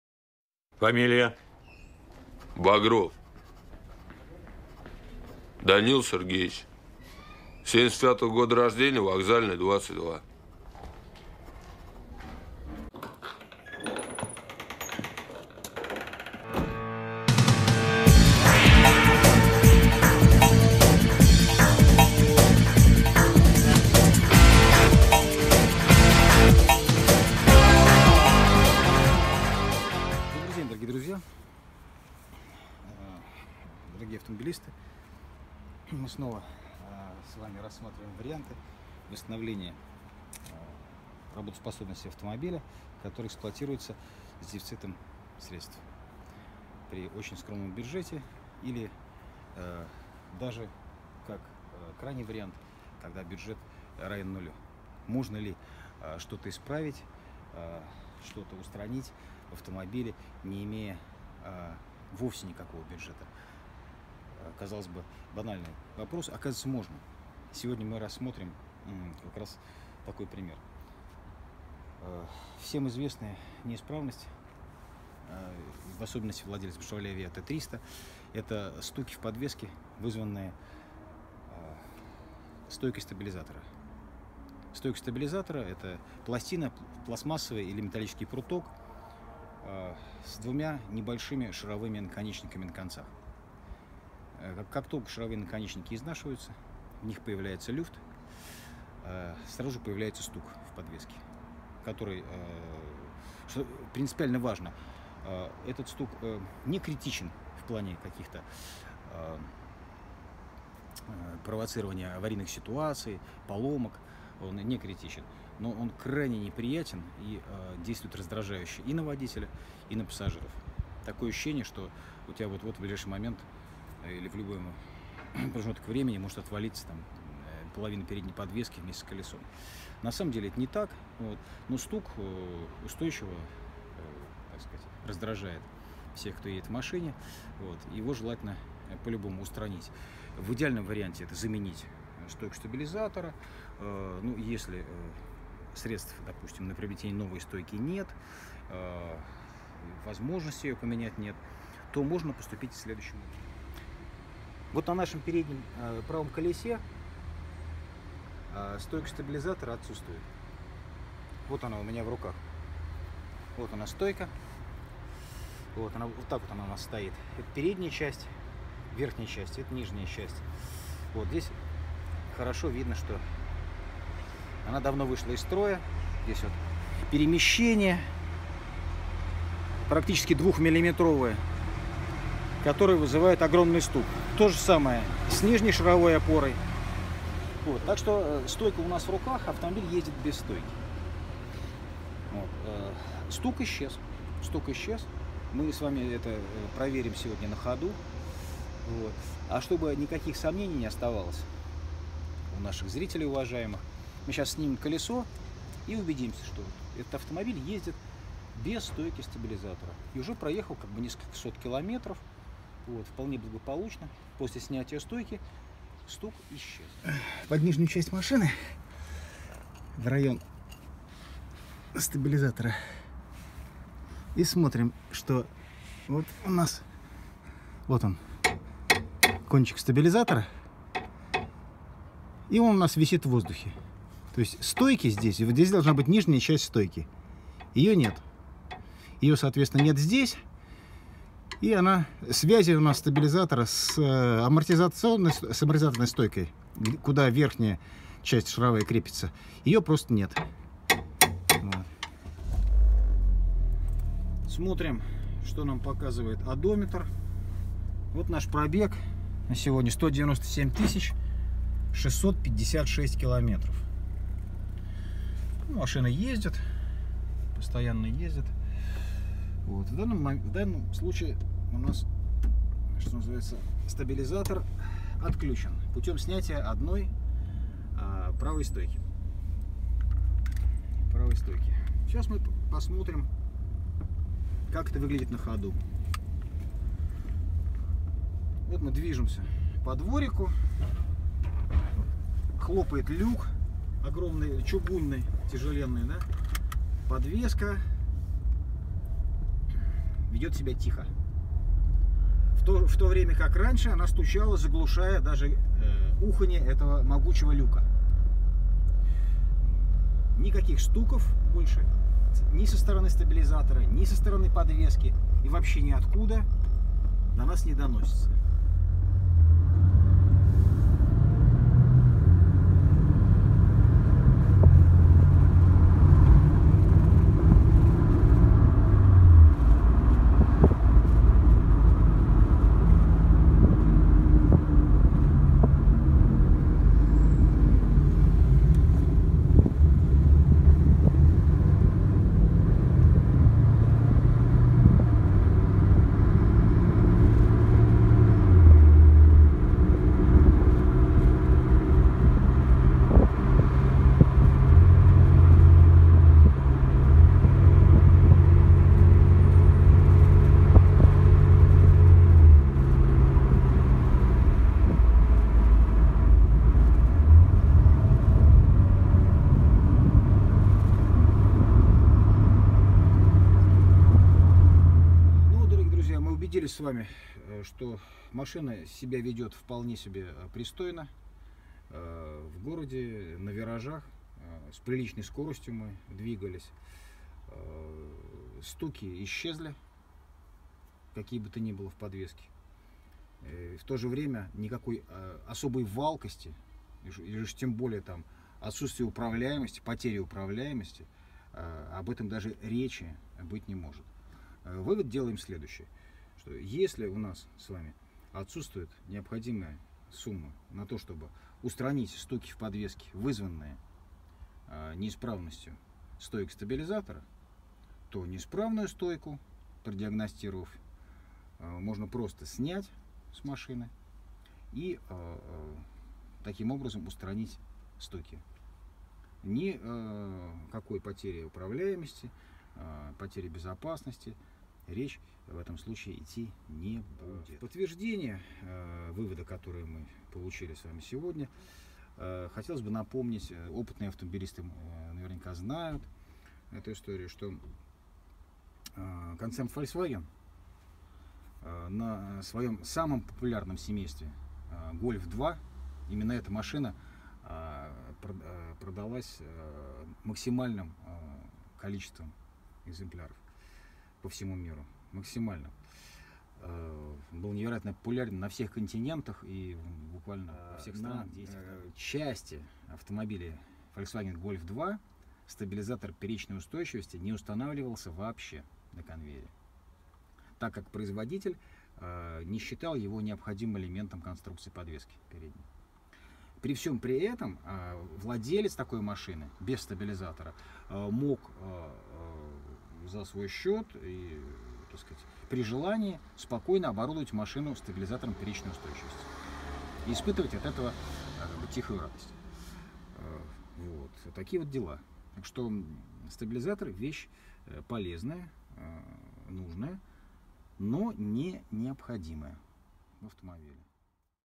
Когда можно не платить за ремонт? Aveo T300, 1,6, АТ, 2012 год, 197,5 тыс. км.
Продолжаем рассказ об особенностях эксплуатации автомобиля  Chevrolet Aveo T300 с автоматической коробкой передач. Пробег автомобиля приближается к 200 тыс. км. Задача прежняя: поддерживать безопасный уровень технического состояния автомобиля и даже улучшать некоторые заводские параметры машины. Оптимизация финансовых вложений при большом пробеге авто.